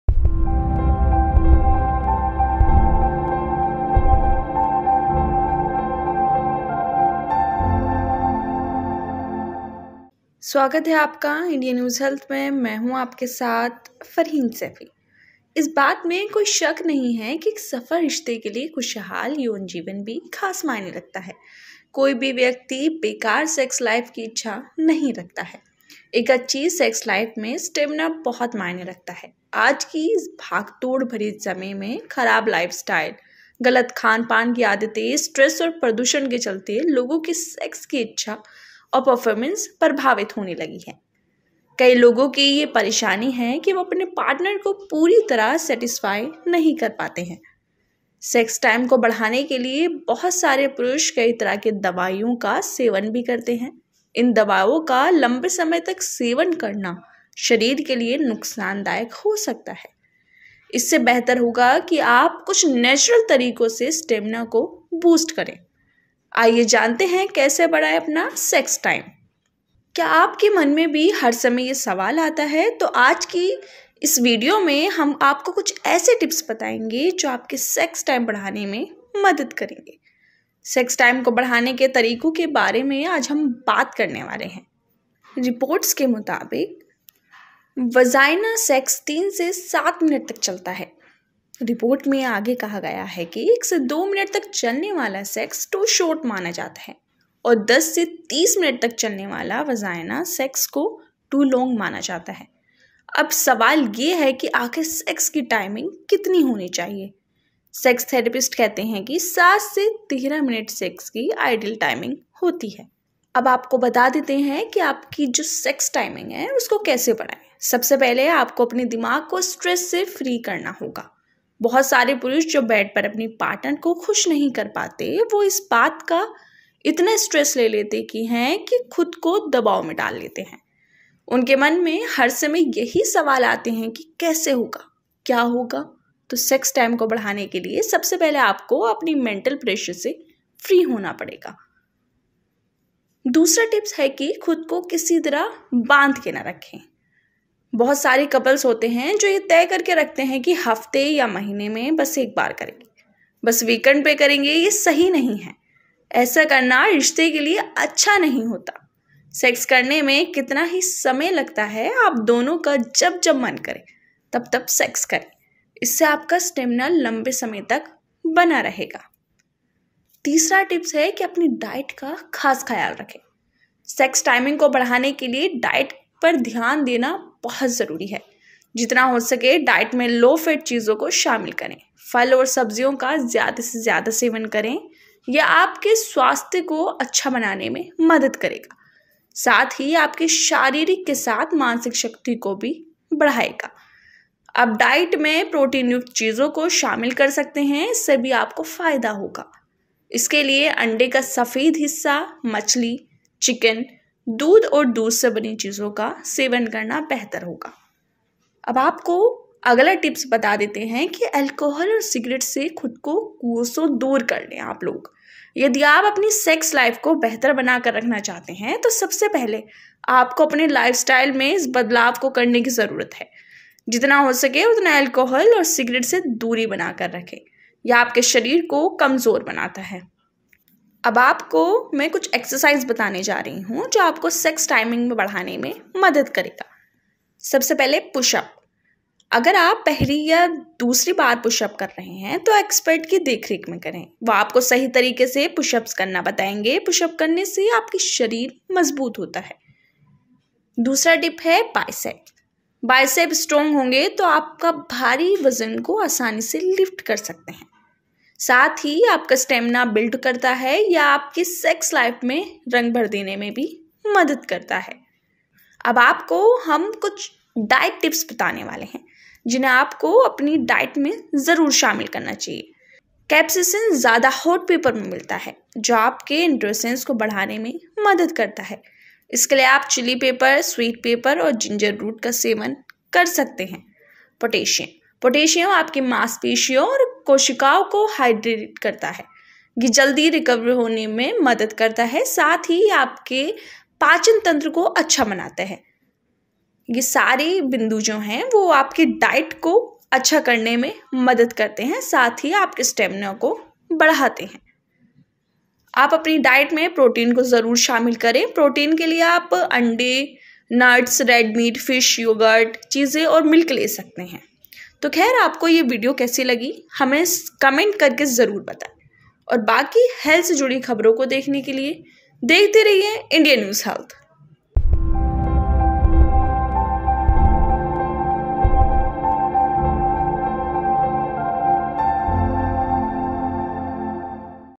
sex: Sex drive has become weak, boost it सेक्स ड्राइव हो गई है कमजोर, इन तरीकों से करें बूस्ट
कैसे बढ़ाएं सेक्स टाइम’ क्या आपके मन में भी हर समय यह सवाल आता है तो आज इस लेख में हम आपको कुछ ऐसे टिप्स बताएंगे जो सेक्स टाइम बढ़ाने में आपकी मदद करेंगे। सेक्स टाइम को बढ़ाने के तरीको के बारे में बात करने से पहले जानते हैं सेक्स करने की एवरेज ड्यूरेशन क्या होती है।
How to increase sex time 'Does this question come in your mind all the time, so today in this article we will tell you some such tips which will help you in increasing sex time. Before talking about ways to increase sex time, let us know what is the average duration of sex.